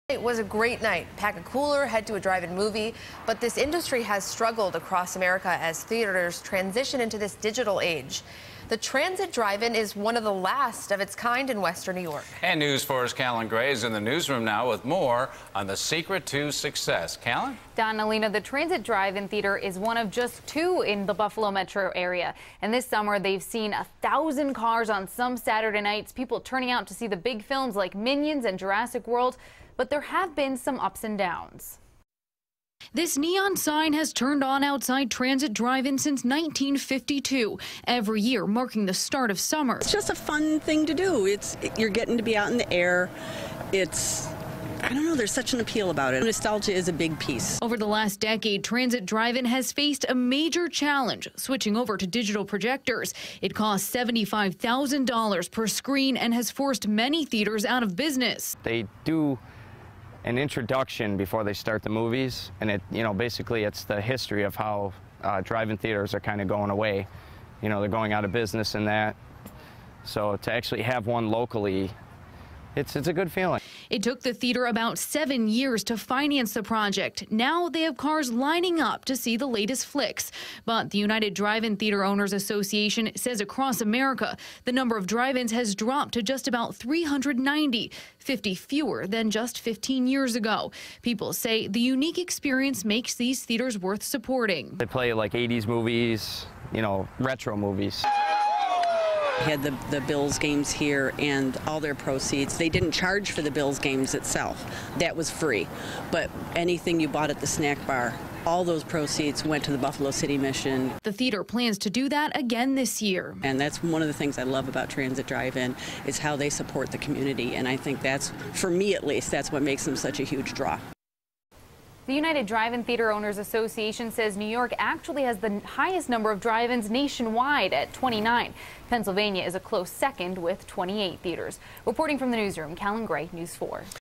Transit Drive-In thrives after switch to digital projectors
The drive-in movie theater industry has struggled across America as theaters transition into the digital age. The Transit Drive-In Theater is one of the last of its kind in Western New York.